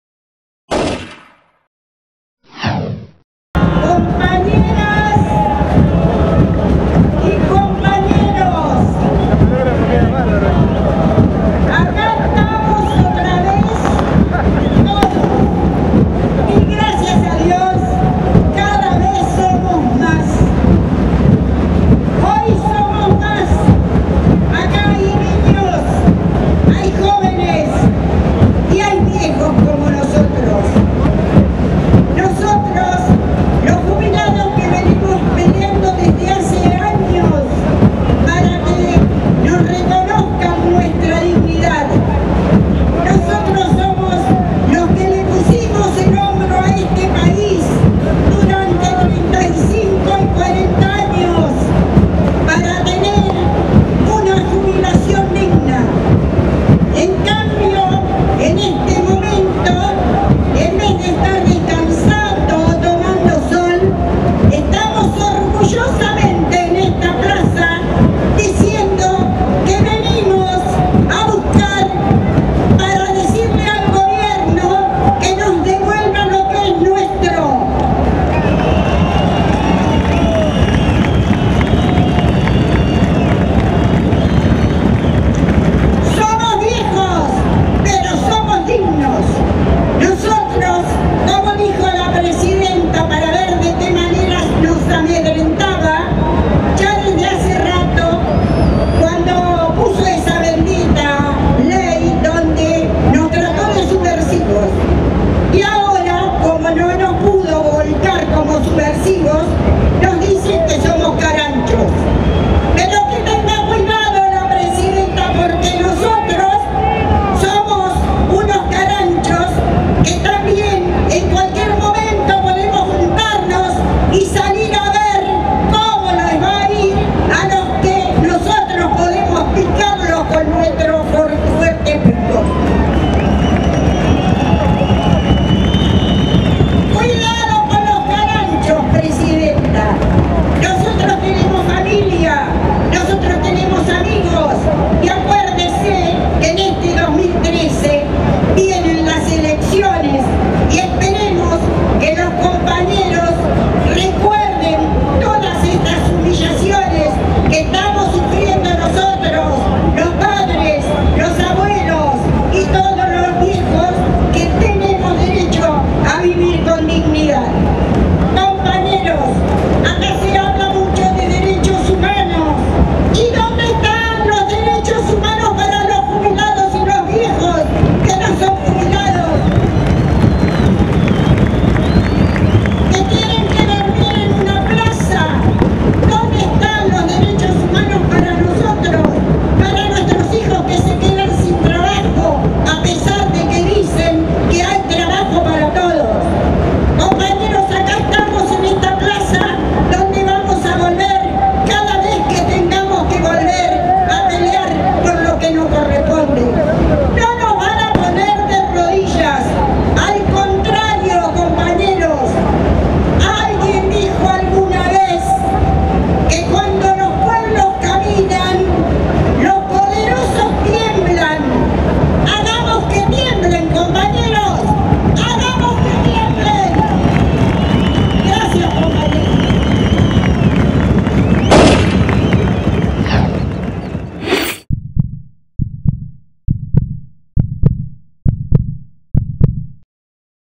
Discurso Lidia Meza - 19 de diciembre de 2012 en Plaza de Mayo
120 mil trabajadores y trabajadoras colmaron la Plaza de Mayo en reclamo de que se cumplan sus derechos y por una Navidad sin hambre y una Patria con justicia social. Ante la plaza repleta, Lidia Meza, secretaria de Previsión Social de la CTA llamó a seguir luchando por lo que les pertenece a los jubilados: el 82% móvil para vivir dignamente.